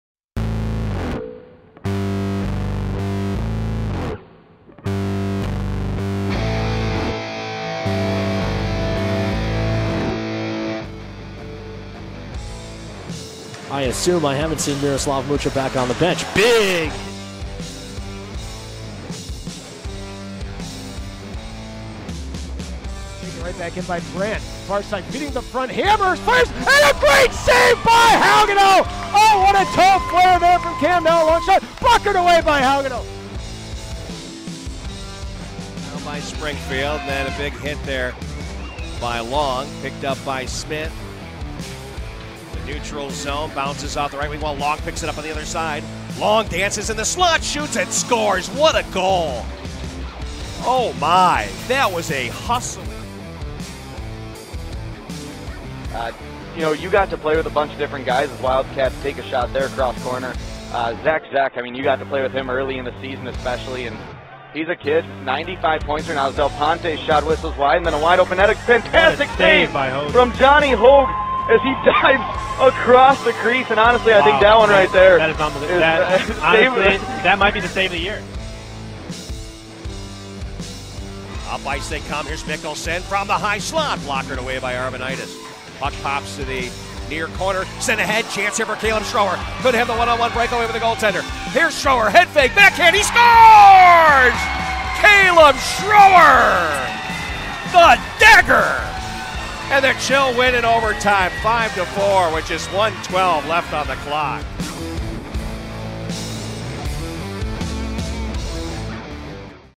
Top 5 Plays of the Week
5. #25 Jake Dittel, Minot Minotauros
4. #45 Camden Haugenoe, Bismarck Bobcats
3. #10 Matt Long, #3 Jordan Muzzillo, #21 Spencer Denbeste, Springfield Jr. Blues
2. #33 Johnny Hogue
1. #16 Caleb Schroer, #3 Steven Quagliata, Coulee Region Chill